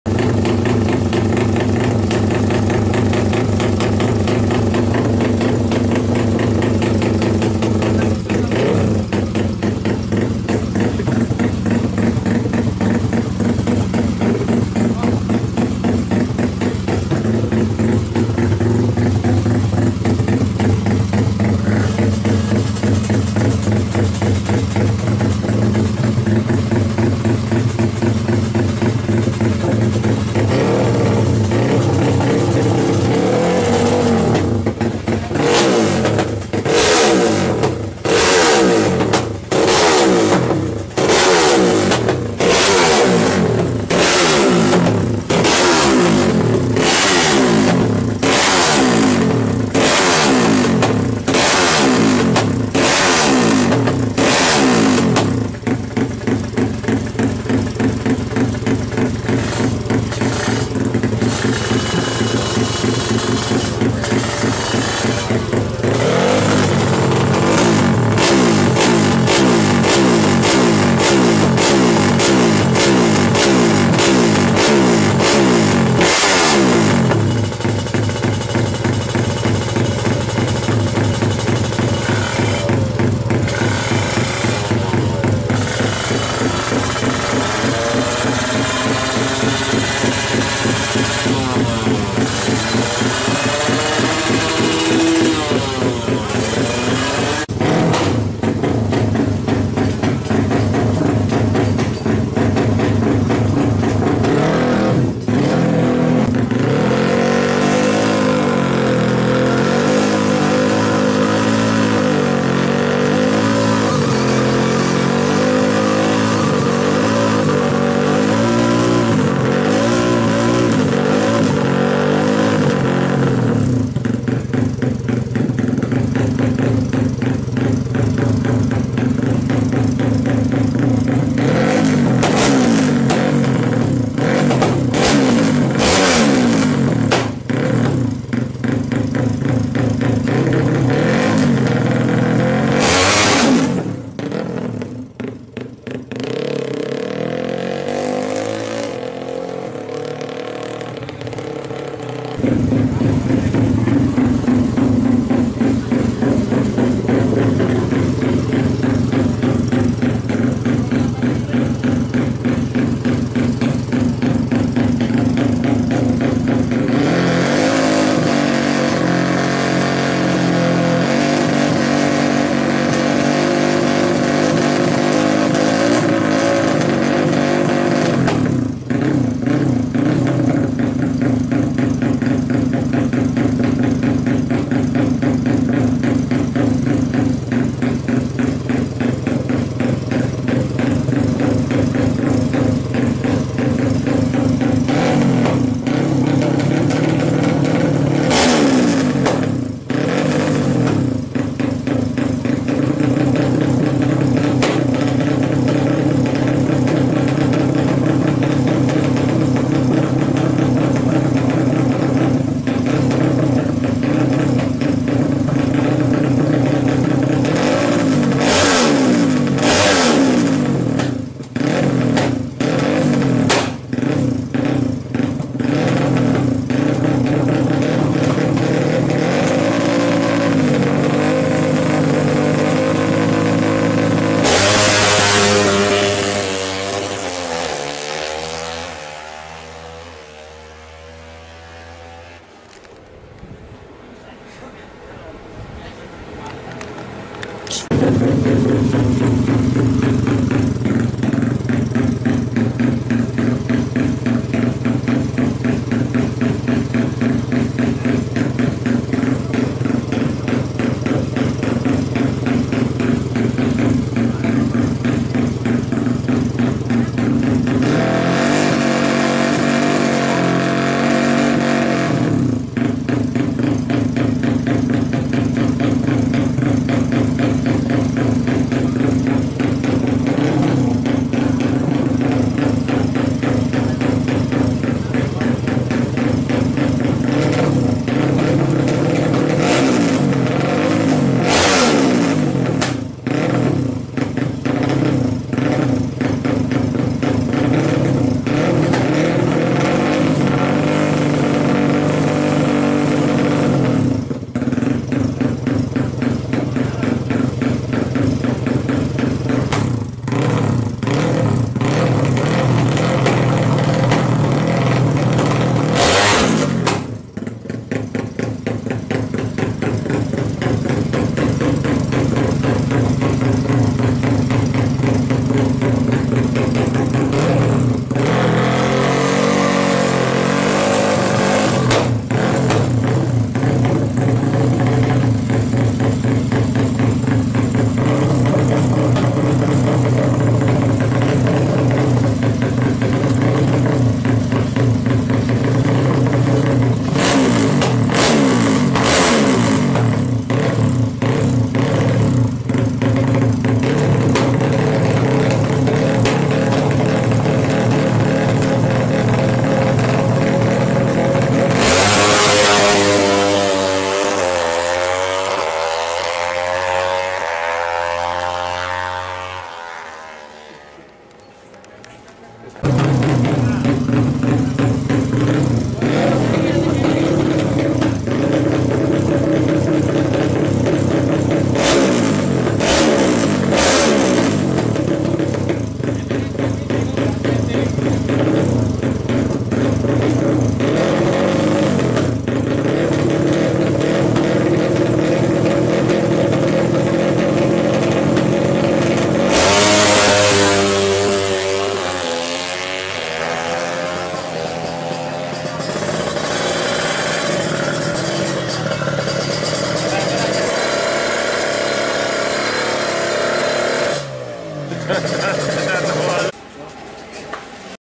NOSTALGIA!! ADIT COCO PERTAMA X SETTING MX FFA 350 CC INJEKSI ABAKURA SOLO. GDS STREET RACING
ADIT COCO WIJAYA KEDIRI PERTAMA KALI NAIK MX FFA 350 CC GARAPAN WAWAN ABAKURA SOLO DI SIRKUIT GDS GANTIWARNO KLATEN